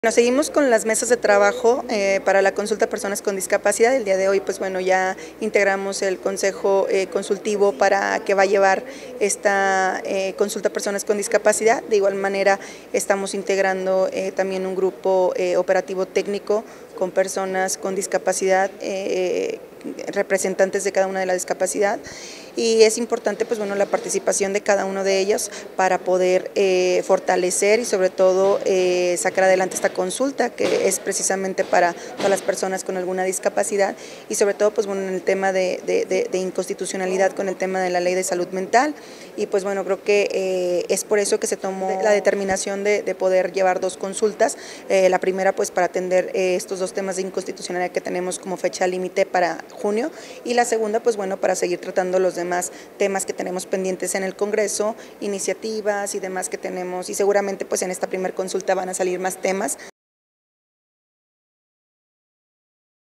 CongresodelEstadoSLP | Dip Cinthia Verónica Segovia Colunga
Se conformó el Consejo Consultivo para el Desarrollo de la convocatoria y la consulta a personas con discapacidad

Tendrá como responsabilidades el acompañar los procesos de elaboración de diseño de la metodología para la consulta a personas con discapacidad: Dip. Cinthia Verónica Segovia Colunga